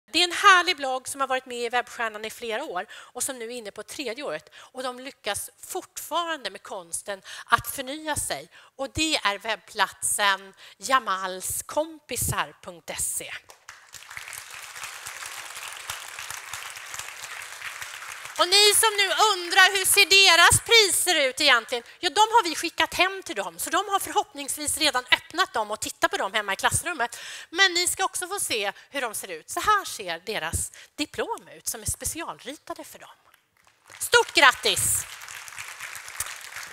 Årets Veteran i Webbstjärnan 2015
Årets veteran i Webbstjärnan 2015 är Jamalskompisar.se